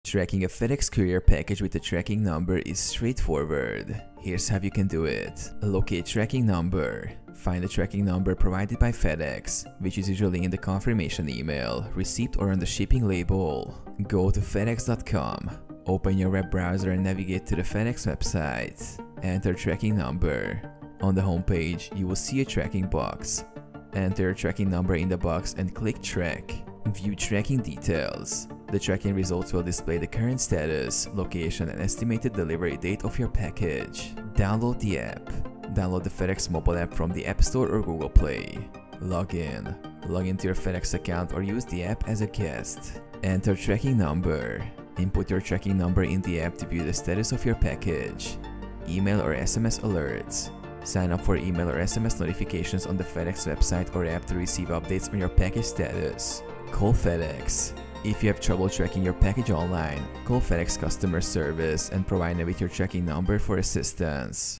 How To Track FedEx Courier With Tracking Number? (2024)
In this video I will explain How You Can Track FedEx Courier With Tracking Number In 2024.